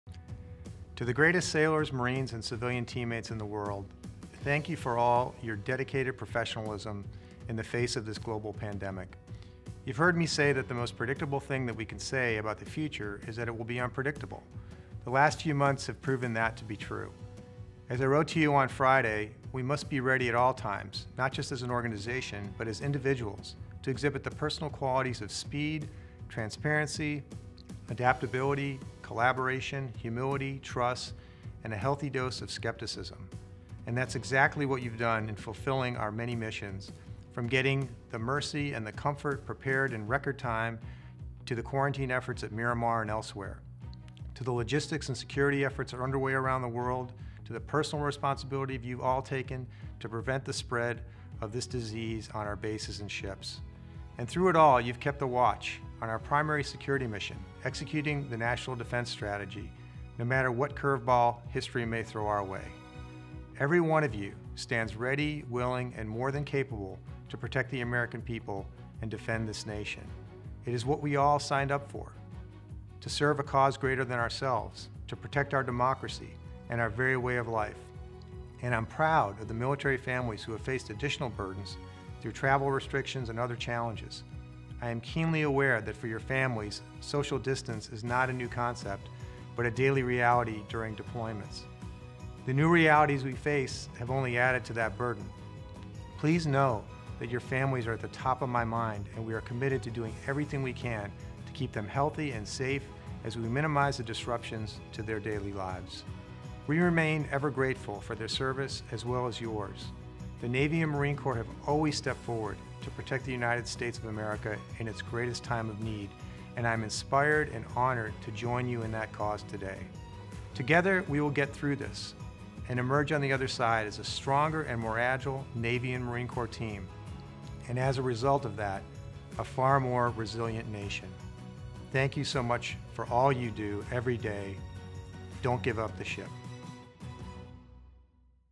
Acting SECNAV Message to the Fleet about COVID-19
Acting Secretary of the Navy Thomas B. Modly addresses @USNavy Sailors, Marines and civilian teammates regarding the ongoing efforts to combat COVID-19 while maintaining operations around the globe.